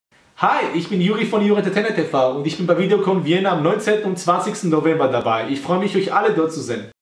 JuriEntertainerTV kommt zur VideoCon Vienna 2016
Holt euch euer Autogramm und ein Selfie mit JuriEntertainerTV bei der VideoConVienna2016!